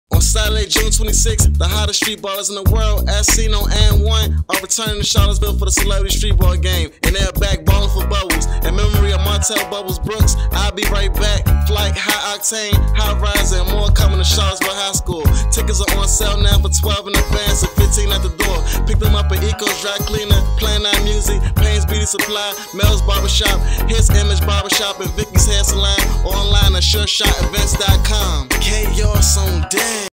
BALLIN FOR BUBBLES CELEBRITY BASKETBALL GAME.mov
The New Legends of Streetball Game is back and returning to Charlottesville, Virginia in memory of a fallen streetball, Martell "Bubbles" Brooks.  It was his dream to play against streetballers, "I'll Be Right Back", Hot Sauce, High Octane, Syder, Flight and others.  On Saturday, September 19th, 2009 "Bubbles" had collapsed after the first quarter of play and was rushed to the hospital where he was later passed on.  The celebrity streetballers had vowed to return in his honor and Saturday, June 26 is the date.